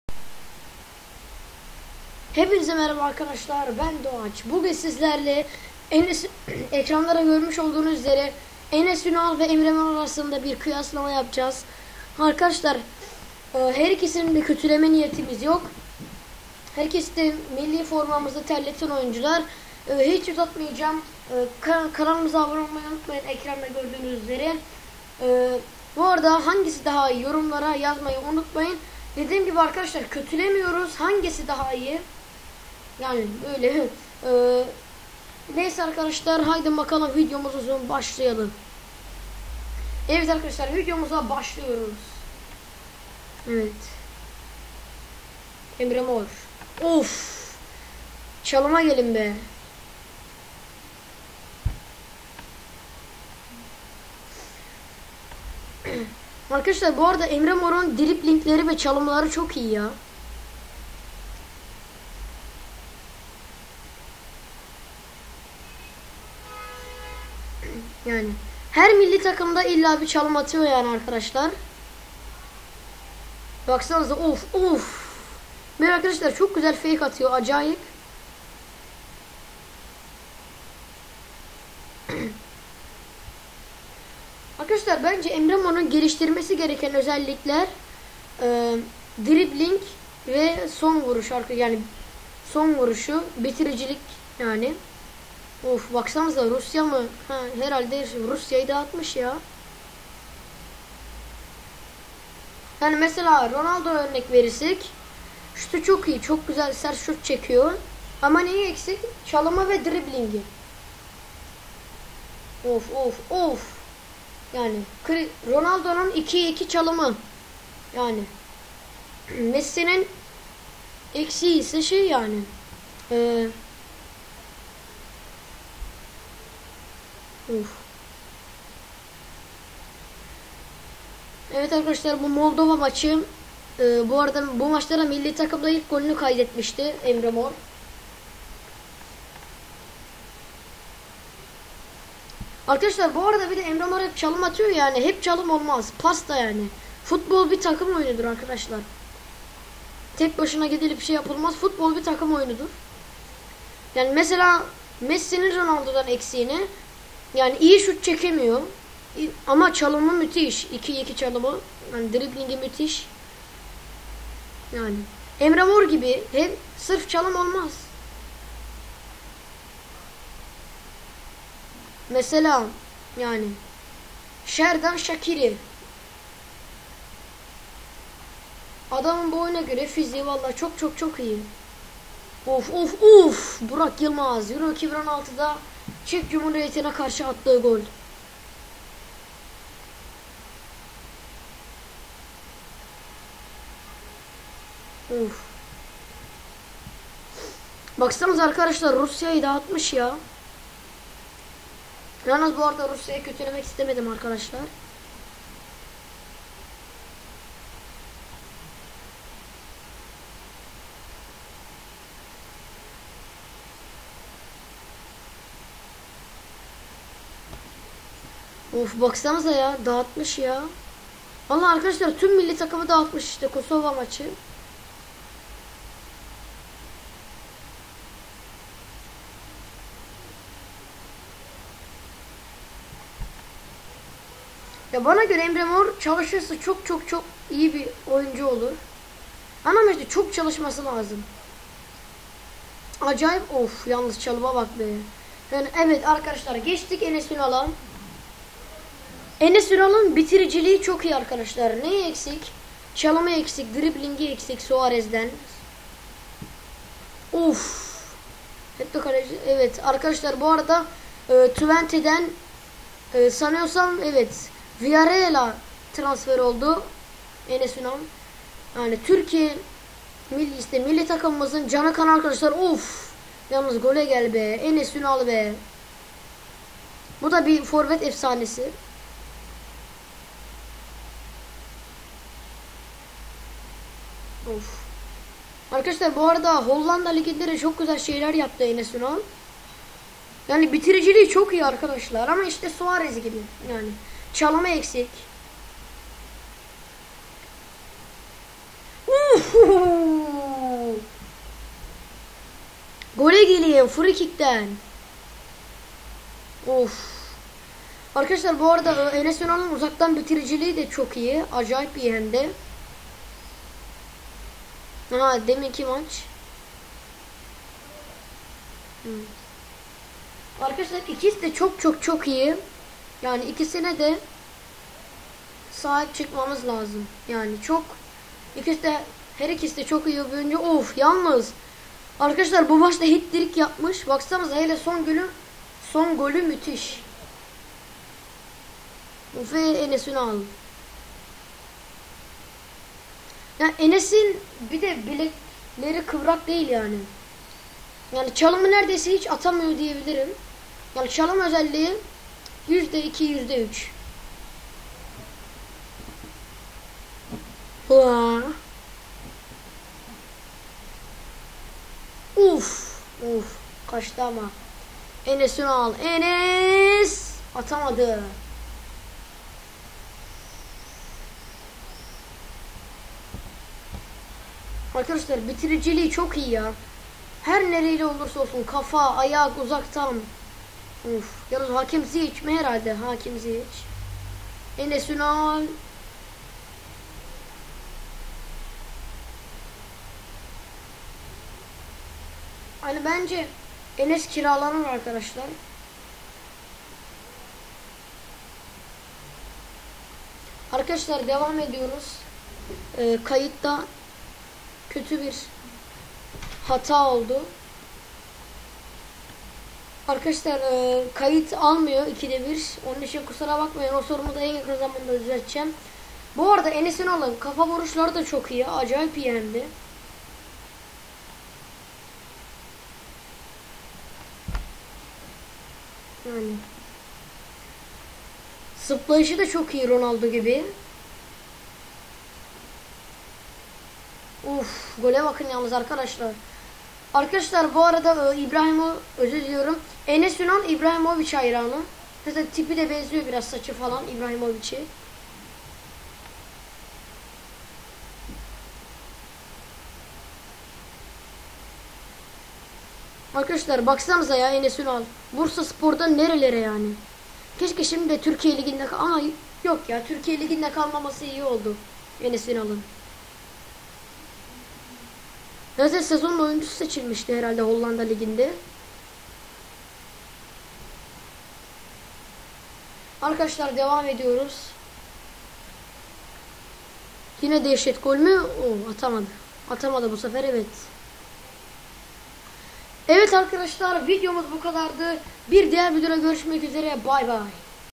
ENES ÜNAL VS EMRE MOR - KİM DAHA İYİ ? - 2017 HD
Hepinize merhaba arkadaşlar ben Doğaç.Bugün sizlerle Emre Mor ve Enes Ünal ' ı kıyasladık arkadaşlar her ikisini de kötüleme niyetimiz yok.Çünkü; her ikisi de milli formamızı terlettiler.İyi seyirler...

NDNG BENİ TAKİP EDİN !

Arkadaşlar sizler için sayfayı yeni açtığımdan fazla paylaşım yapamadım ama ileride çok güzel paylaşımlar yapacağım...